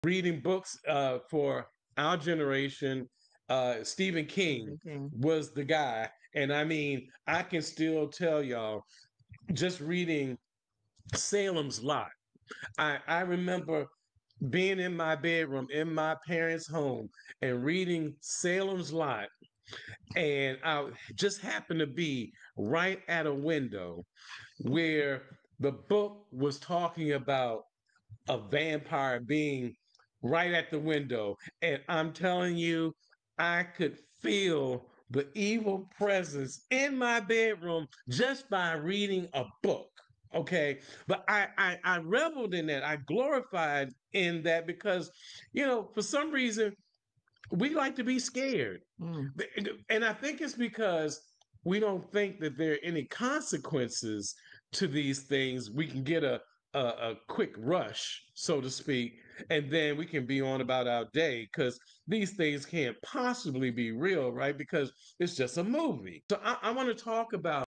Why We LOVE Being Scared by Horror Movies!
Join us for a deep dive into the allure of horror in entertainment! SPEAKER_01 shares personal tales of fear and fascination with horror films and literature, exploring their impact on our psyche and the thrilling rush they offer. Discover the blend of faith and fright in cinematic experiences!